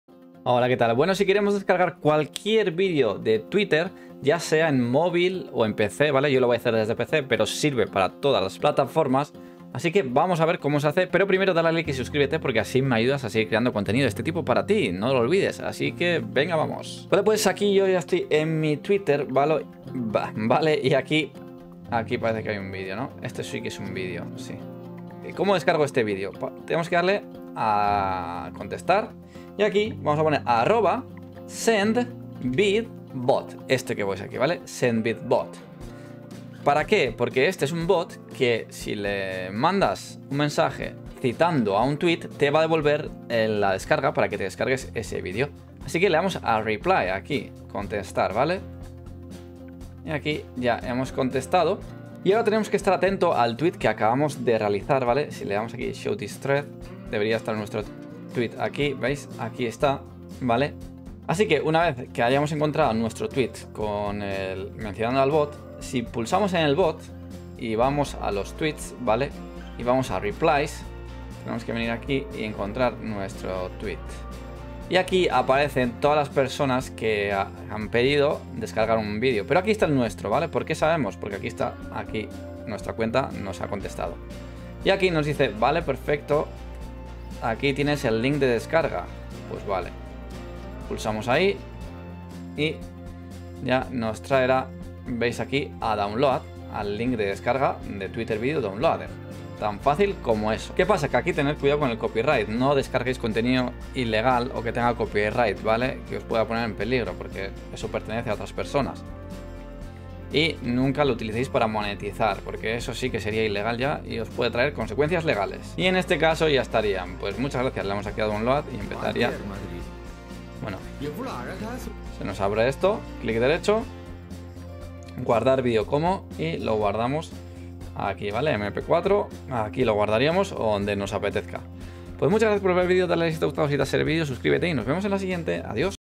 Cómo descargar videos de Twitter en cualquier plataforma
¿Quieres aprender a descargar videos de Twitter? En este video te mostramos cómo hacerlo en unos sencillos pasos. Aprenderás a usar herramientas en línea para descargar videos directamente desde Twitter.